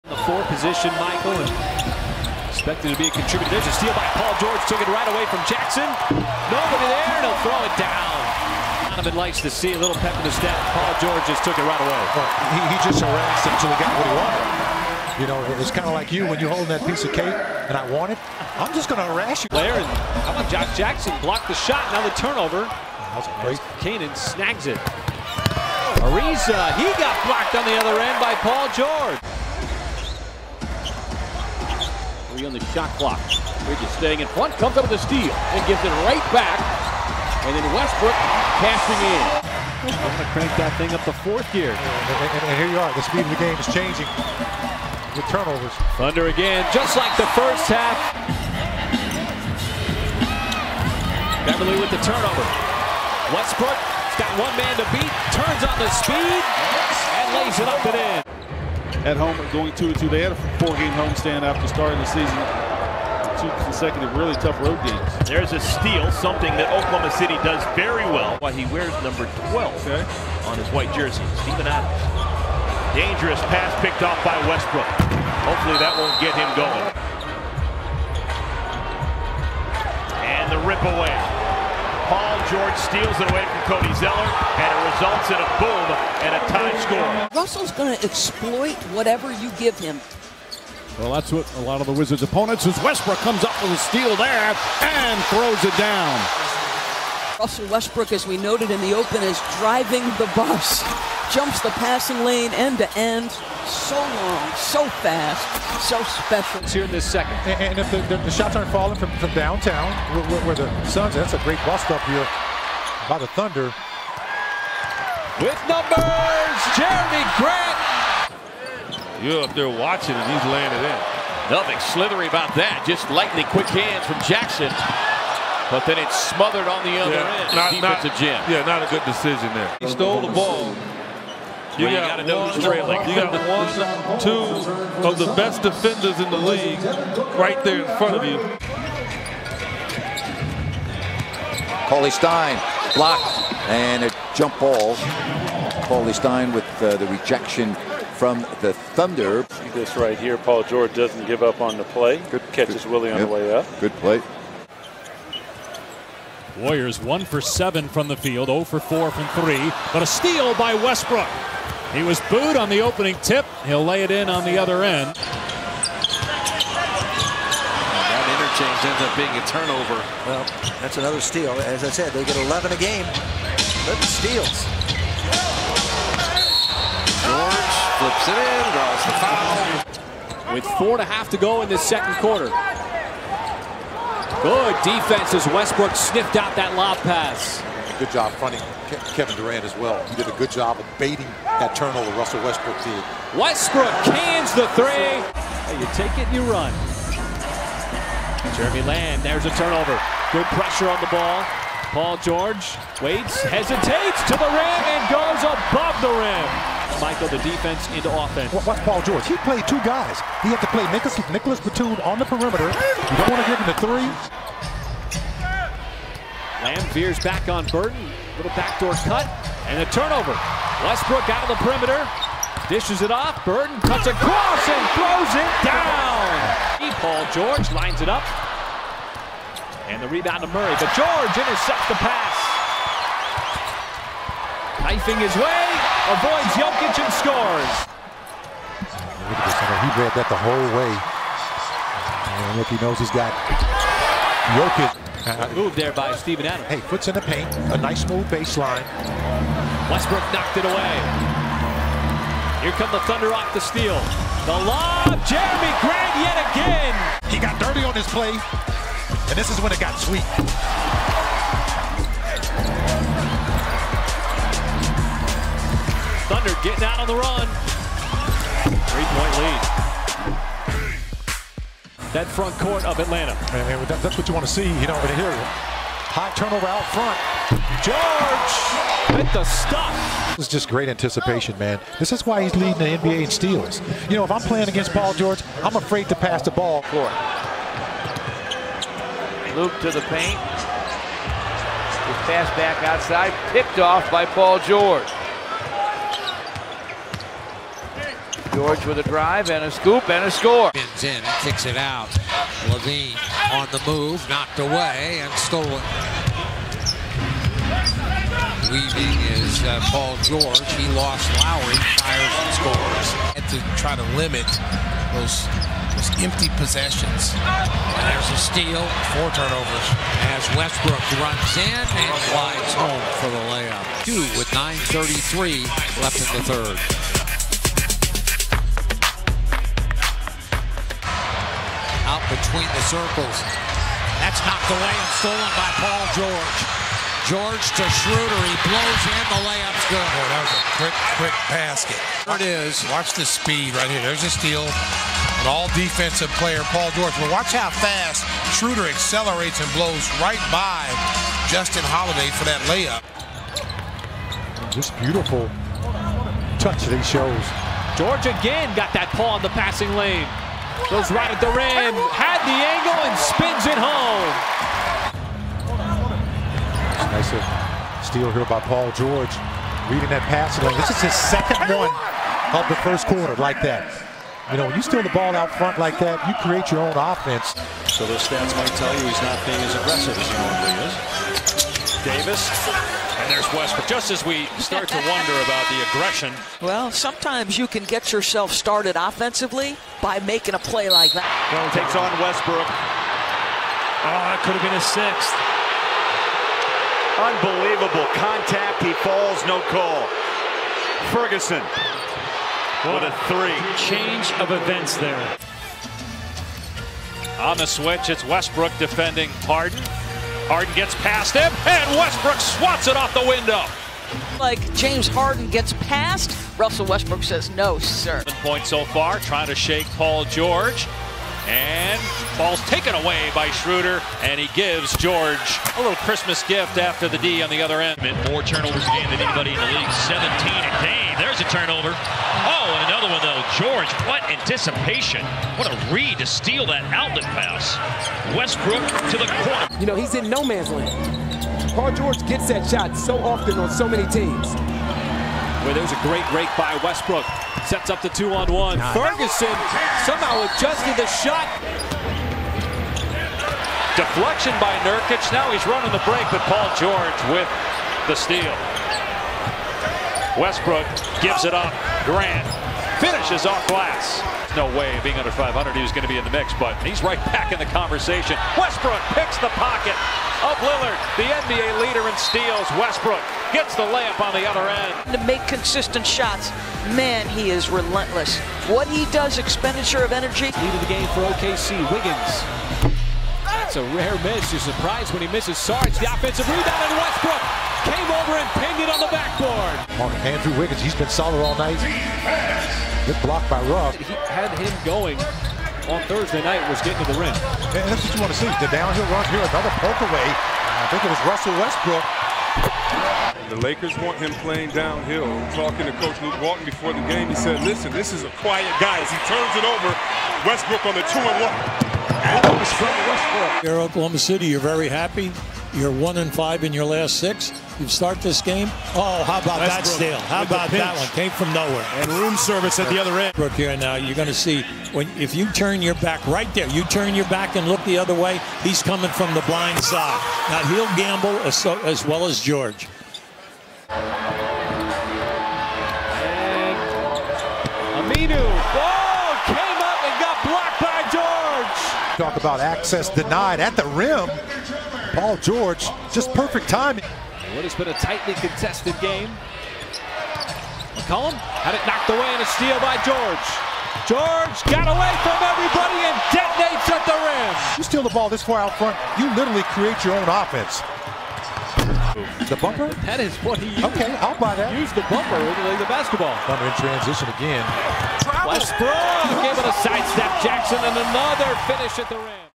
Paul George and Russell Westbrook are LOCKING UP | 2018-19 OKC Defense Mix
PG-13 and Russ are the NBA's top two leader in steals. Both are averaging career-highs and have lifted OKC into one of the league's best defensive units.